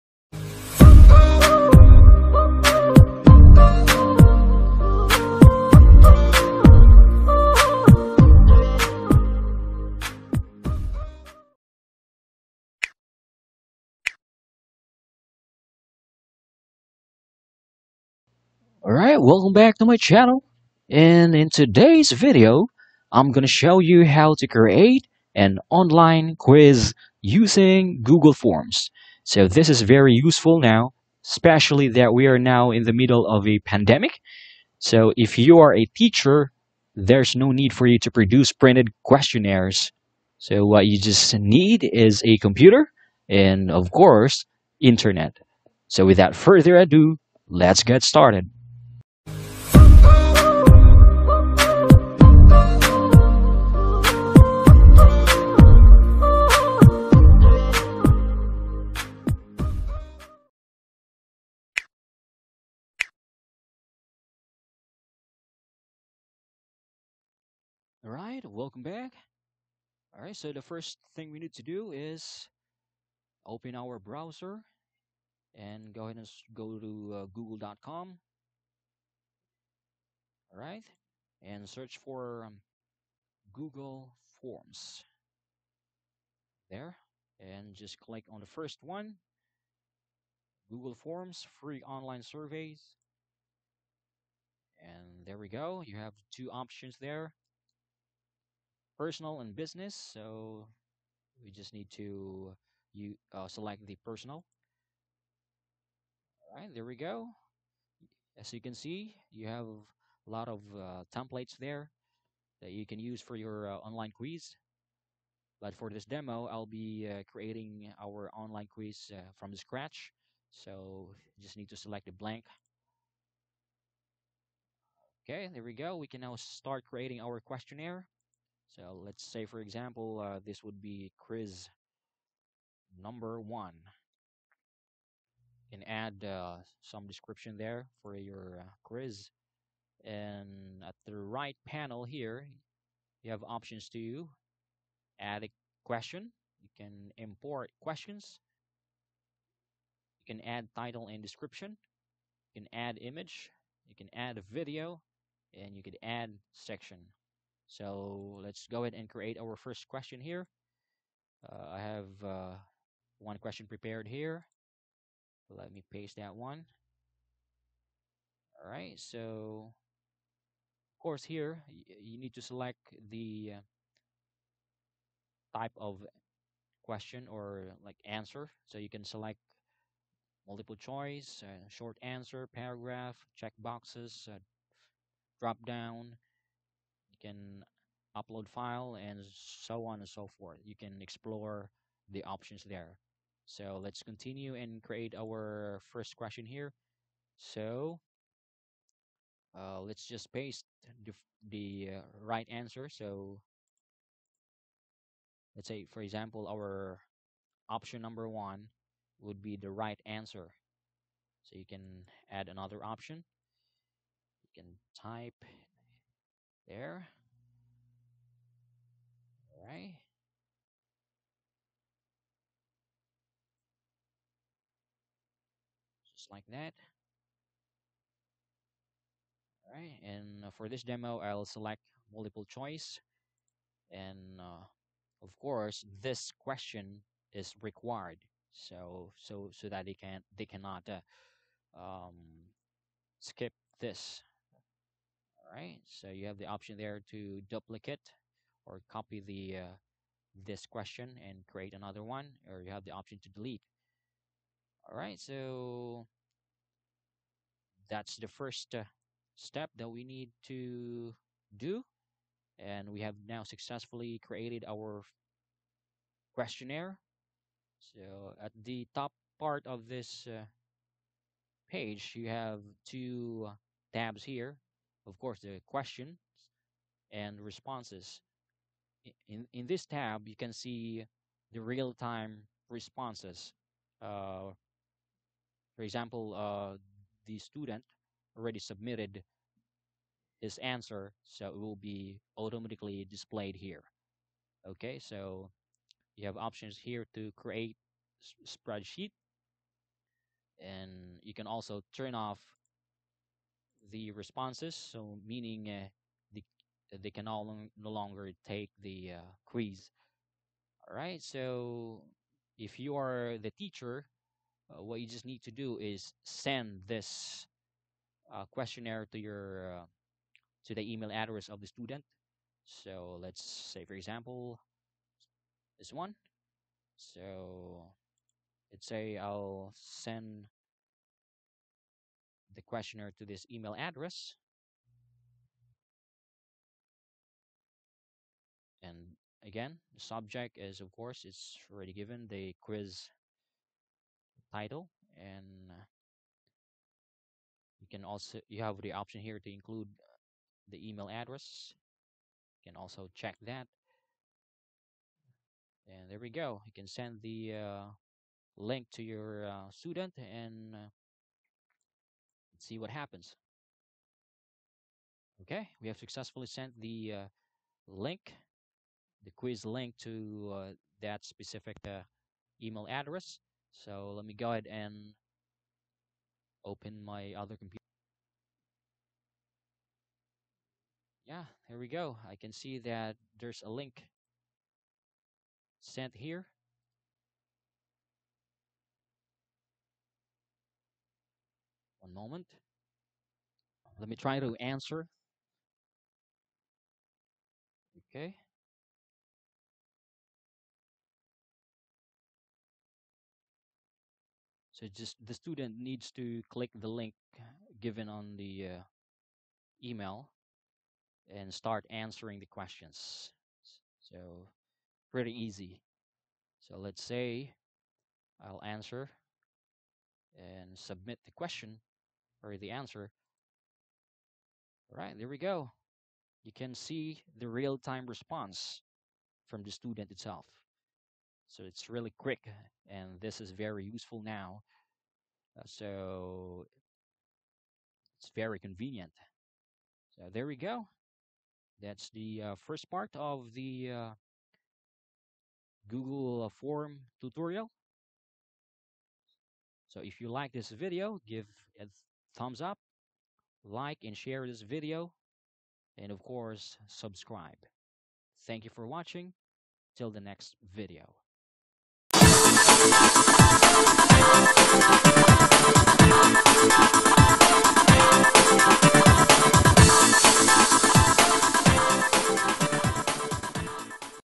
Free Online Quiz Maker for Teachers
This is a simple demo on how to create an online quiz for free using google forms. This is very useful for the teachers in adapting to the new normal "Online Class".